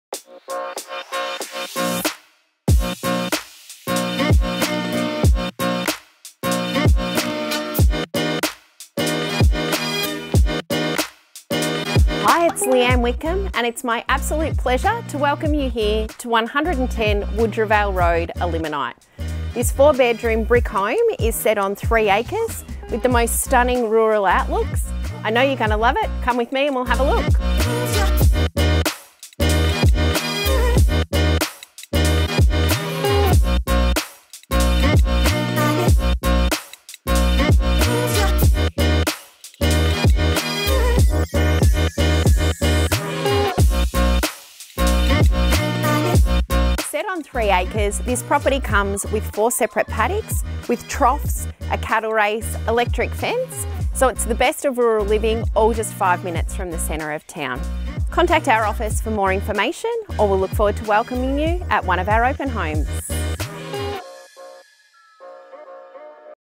Just Listed - 110 Woodrowvale Road, Elliminyt
Nestled atop a serene three acre  plot in Elliminyt, this captivating brick veneer residence epitomizes the essence of rural living, boasting panoramic countryside views from every vantage point. It's the perfect blend of a picturesque setting and a family-oriented design, offering a lifestyle many dream of.

The layout is exceptionally welcoming, centred around a spacious living area that exudes a warm, country charm. The highlight of the living room is undoubtedly its cathedral ceilings and an exposed brick backdrop to a large wood fire, promising cosy winter gatherings. During the warmer months, a reverse cycle air-conditioner provides a cool retreat. New carpeting and blinds throughout will also help with the comfort and climate inside all year round. This space also allows for a study area for students and home workers alike.

Culinary enthusiasts will delight in the modern, well-appointed kitchen, featuring an electric wall oven and plentiful storage and counter space. The kitchen is flanked by a generous dining area, allowing for versatile entertaining and family meal arrangements.

The home includes four well-proportioned bedrooms, each with built-in wardrobes and ceiling fans, and one boasts an additional reverse cycle air-conditioner for enhanced comfort. A chic family bathroom, complete with a separate toilet, services the bedrooms.

The home's charm extends outdoors with timber decks on the eastern and western facades, capturing the sublime sunrise and sunset — perfect for enjoying a morning coffee or evening beverage. The well-established gardens provide a secluded haven, harmonizing with the local birdlife and rural vistas.

Additional features include ample storage and workshop space beneath the house, a double garage, and an extra 9x6m concreted, 2 phase powered shed for all your storage needs. The land is broken up in to four paddocks with troughs, electric fencing and a cattle race.  The property also comes with a 99-year lease over an adjoining one-acre allotment of Crown land, effectively expanding your outdoor space to four acres.

Its prime location ensures convenience, with Colac's town centre a mere 5 minute drive, school bus stop nearby, and the scenic Old Beechy Rail Trail for walks or bike rides just minutes away.

We invite you to discover this impressive home for yourself, confident that a single visit will convince you of its allure. This is an opportunity not to be missed.